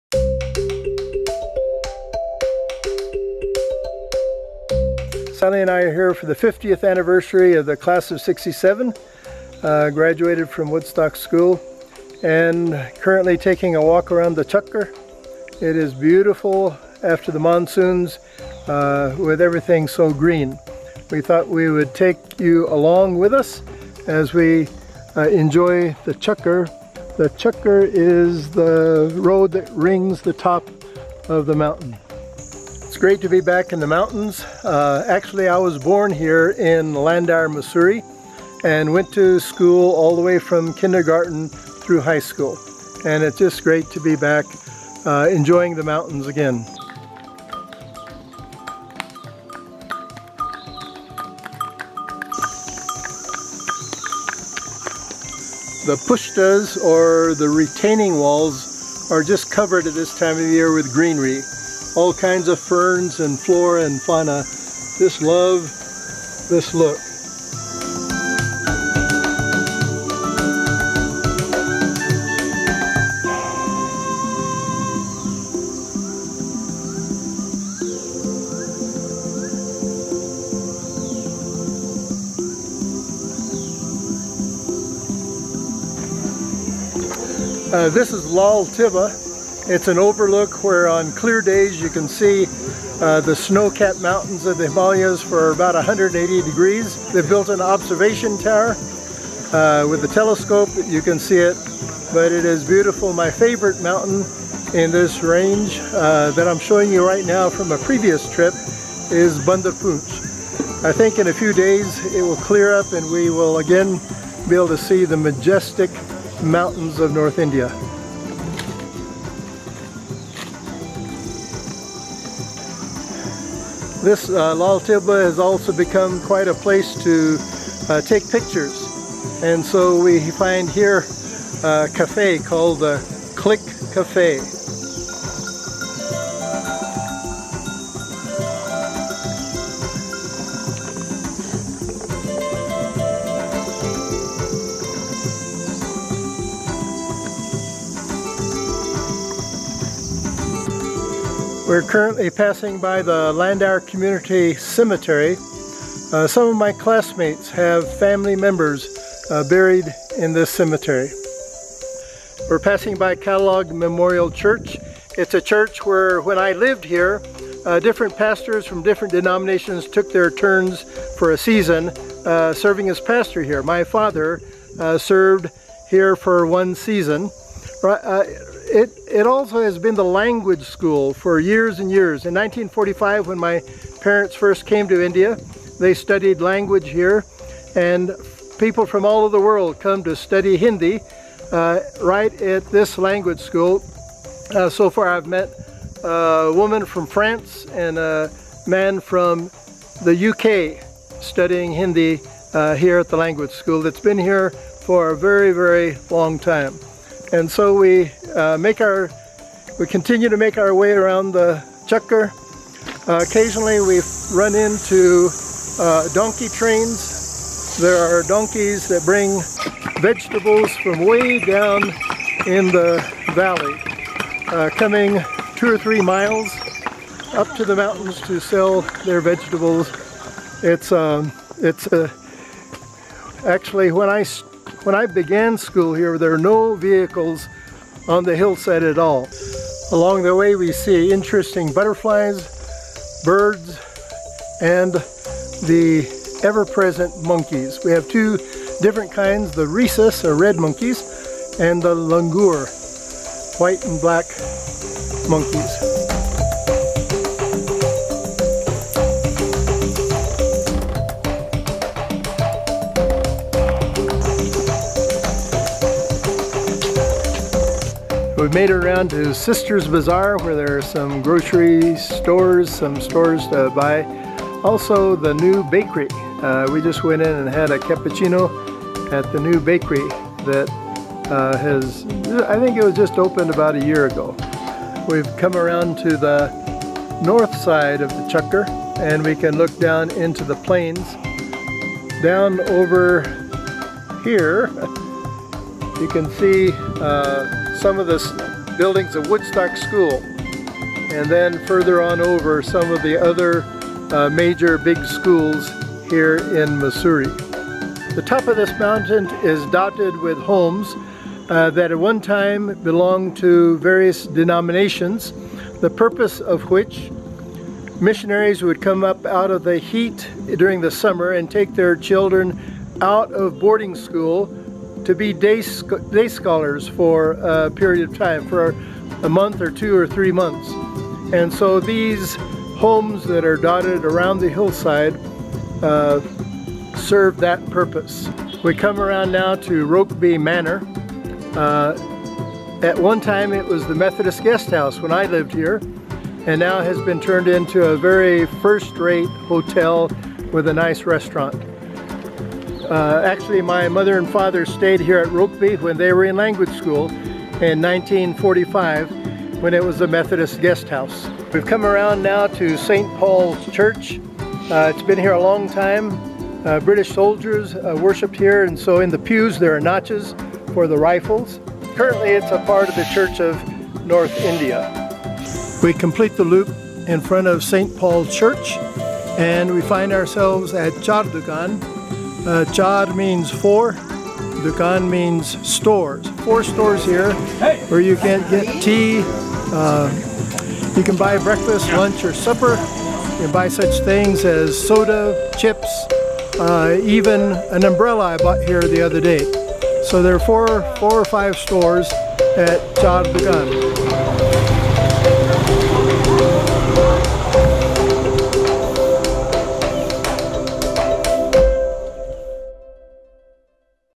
A Walk Around the Chukkar, Landour, Mussoorie, India
Sally and I are in Landour in north India for the 50th anniversary of my Woodstock School high school graduating class.  A number of my classmates have returned.  The chukkar, the road that rings the mountain above Woodstock School, is particularly beautiful after the long monsoon rains.  Come and take a walk with us.